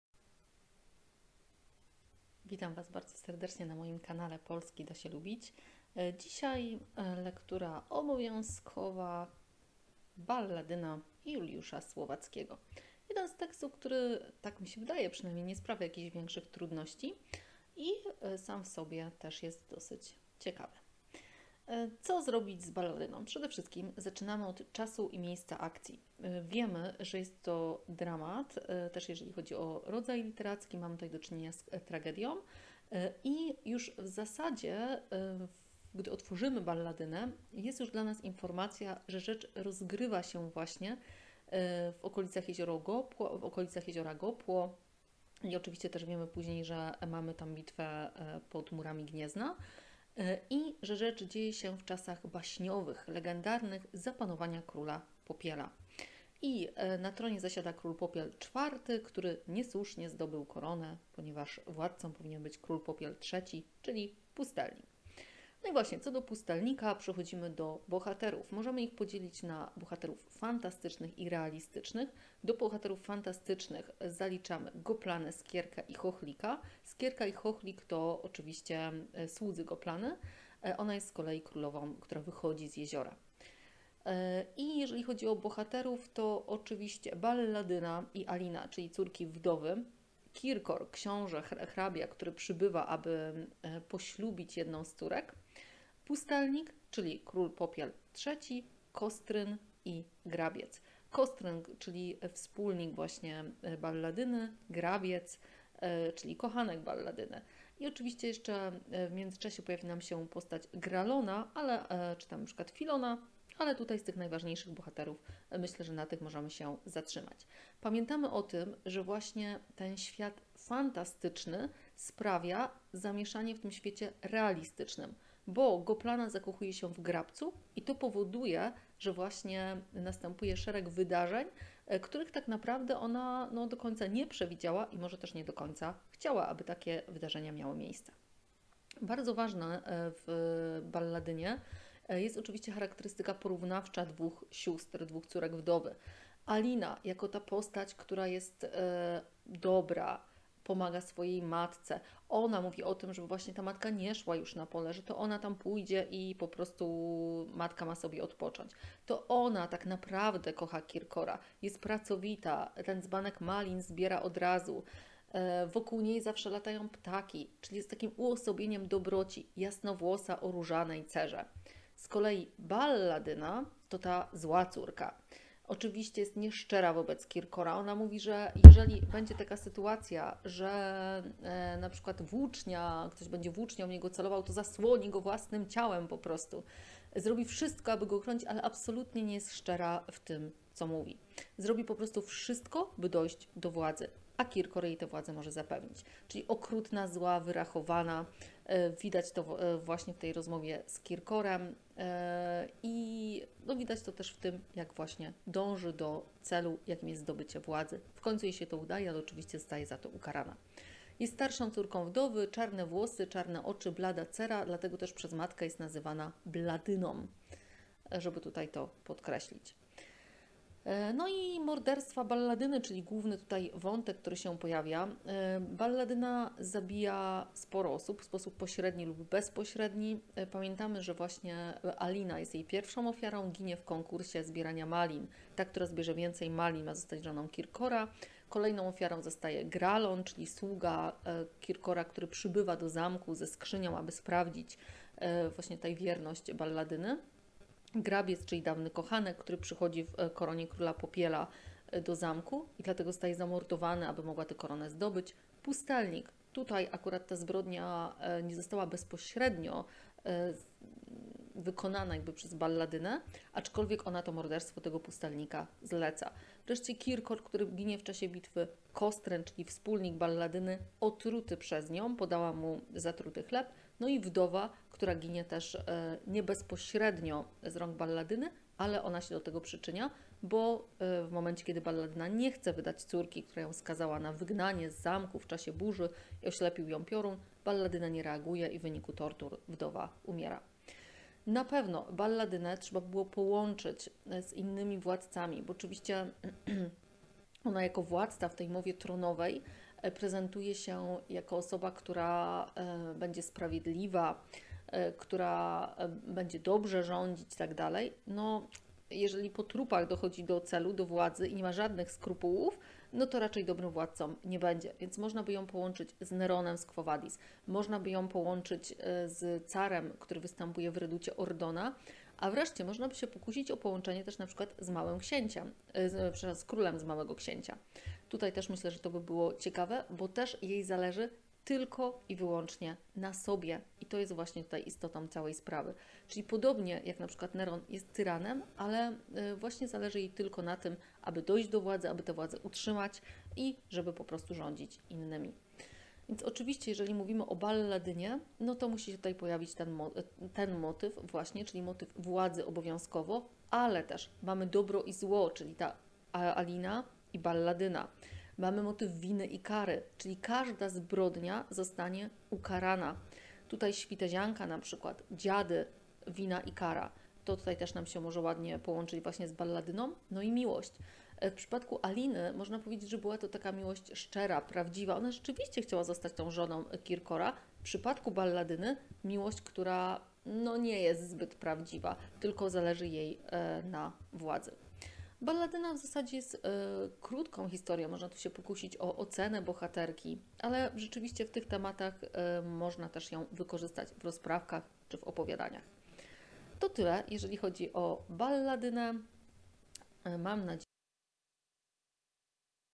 "Balladyna" Juliusza Słowackiego
"Balladyna" Juliusza Słowackiego - powtórzenie wiadomości przed egzaminem ósmoklasisty.